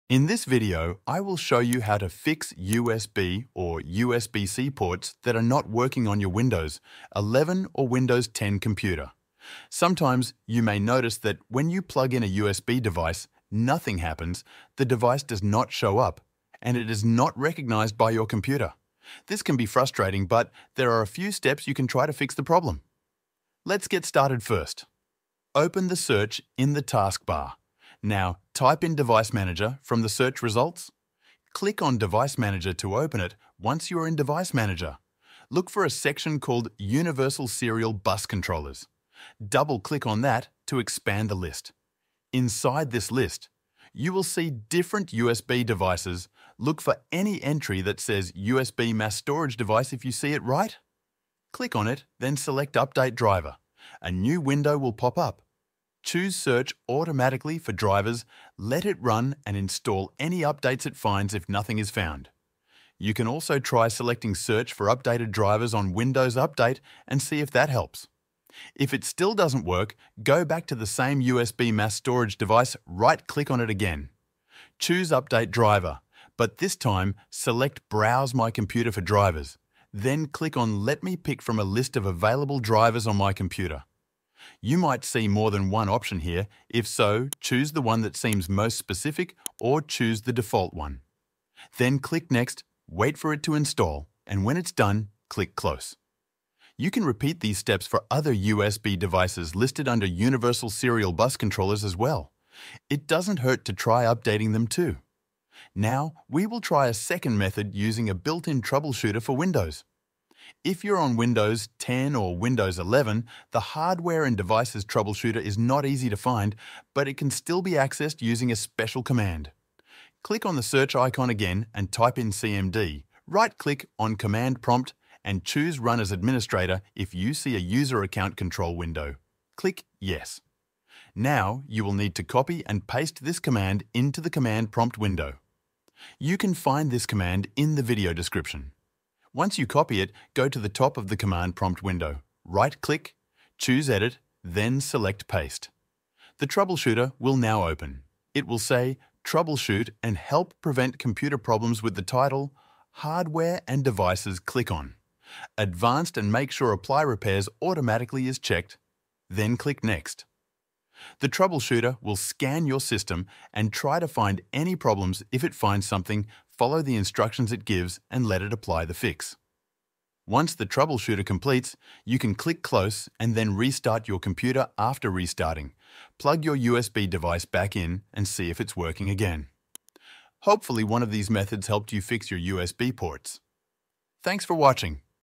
How To Fix USB/USB-C Ports Not Working on Your Windows 11/10 PC [Step Guide]
Are your USB or USB-C ports not recognizing devices on your Windows PC? Whether you're using Windows 10 or 11, this step-by-step tutorial walks you through simple and effective ways to get your USB ports working again. No need for third-party tools—just use the built-in features of Windows!

▶️ What You’ll Learn in This Video:
How to access and use Device Manager to check USB devices
Steps to update or reinstall USB drivers correctly
How to run the hidden Hardware and Devices Troubleshooter
Restart and verification steps to ensure full USB functionality

Command prompt: msdt.exe -id DeviceDiagnostic

Timestamps
00:00 – Intro
00:28 – Open Device Manager
00:51 – Update USB Mass Storage Device driver
01:08 – Manual selection of USB drivers
01:59 – Accessing Hardware Troubleshooter via CMD

Quick solutions for USB issues without needing tech support
Perfect for beginners—no advanced skills required
Works for all common USB and USB-C device problems
Compatible with both Windows 10 and Windows 11

If this video helped you, consider supporting us:
👍 Like the video to help others discover it
🤝 Share it with friends or coworkers facing the same issue